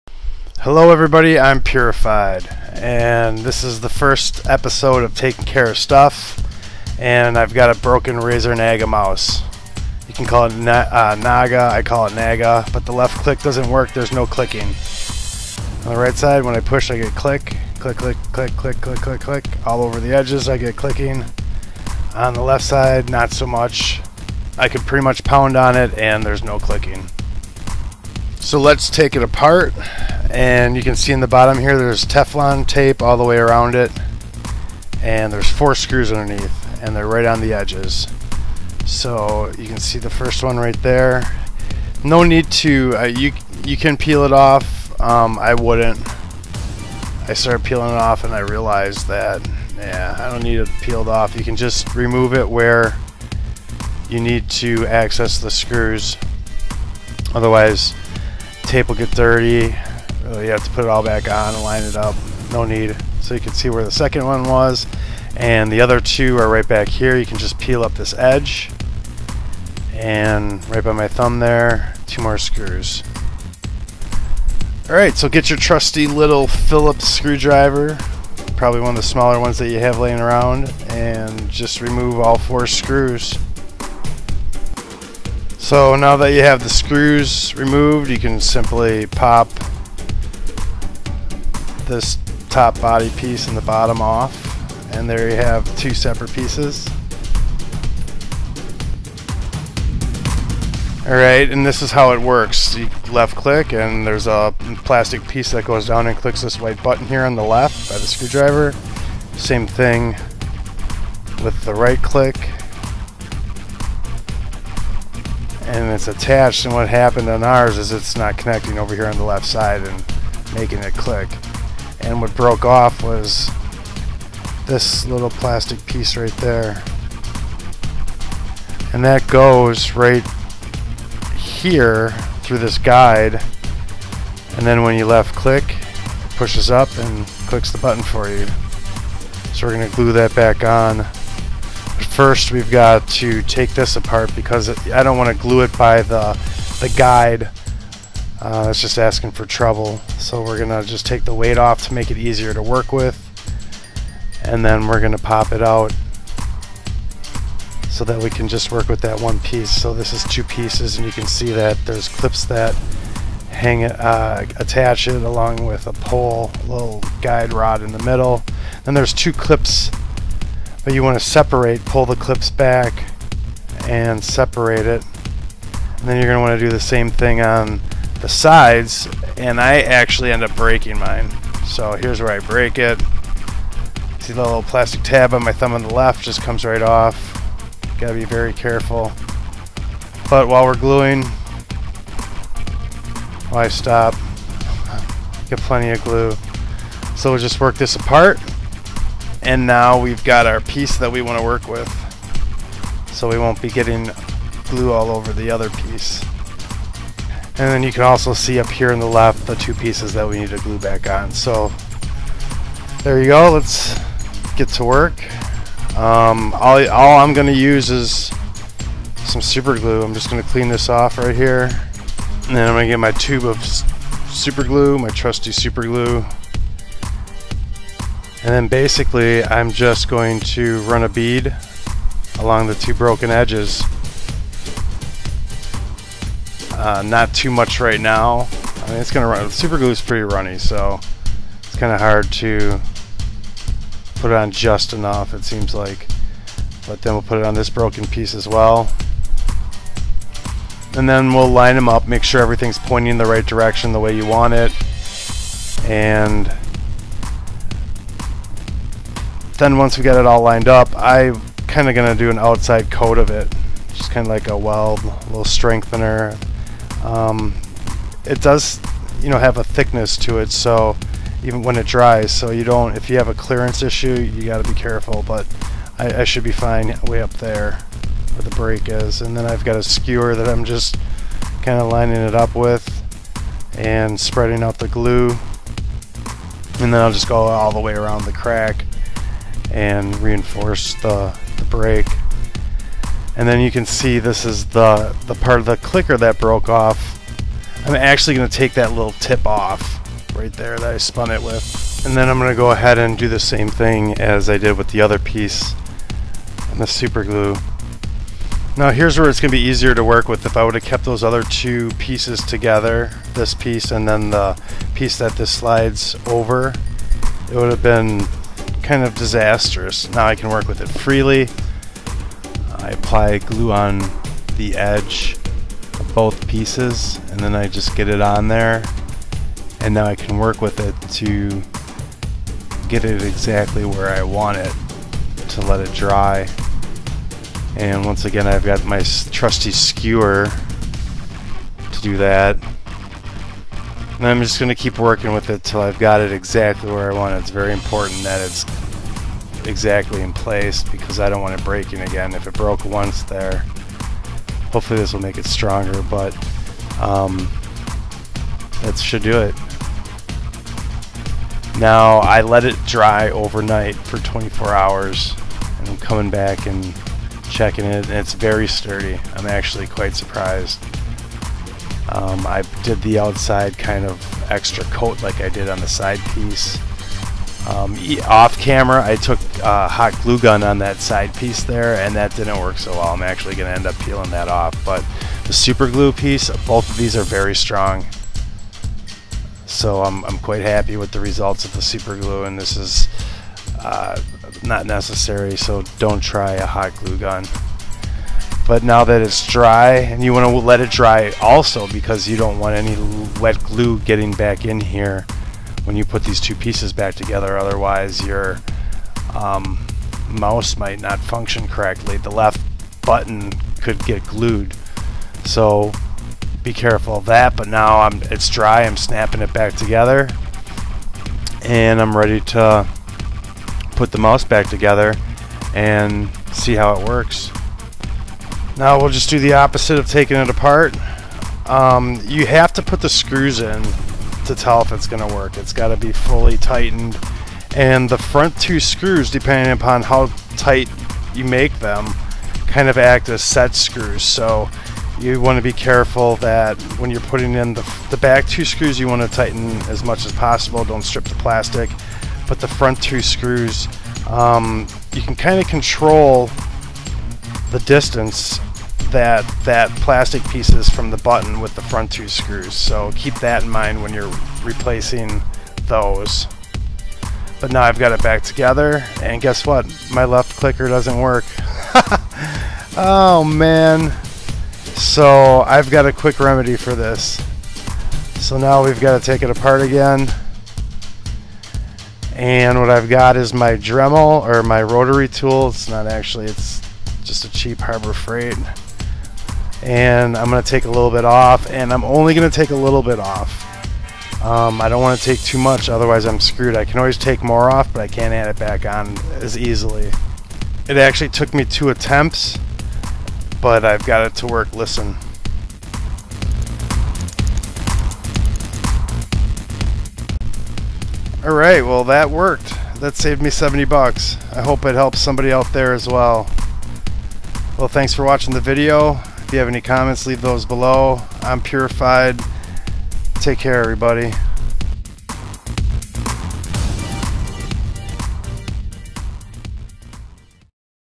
How To Disassemble and Repair Broken Left Click on Razer Naga Mouse
In this video I disassemble my Razer Naga mouse to repair the broken left click mechanism.